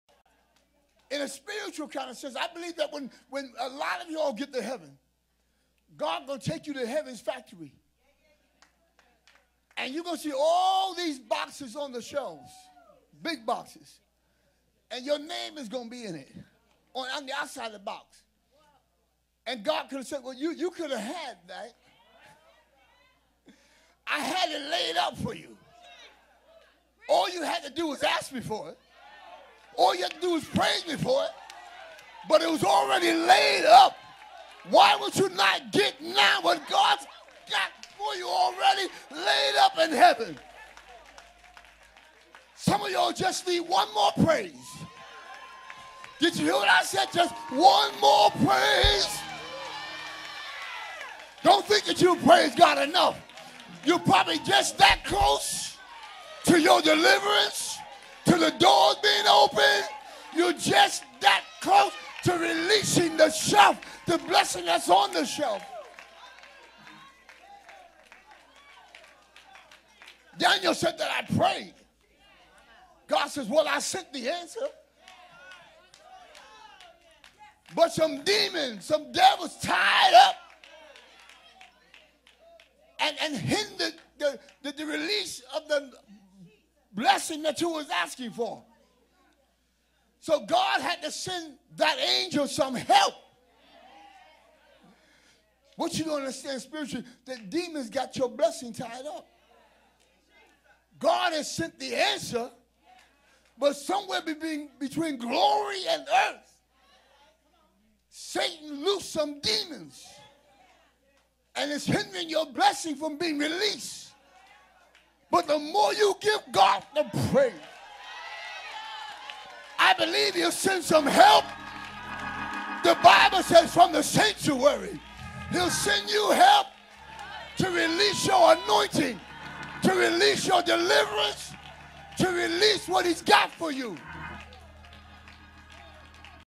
DAYS OF HEAVEN ON EARTH
Bishop Harris preaching at Grace Cathedral International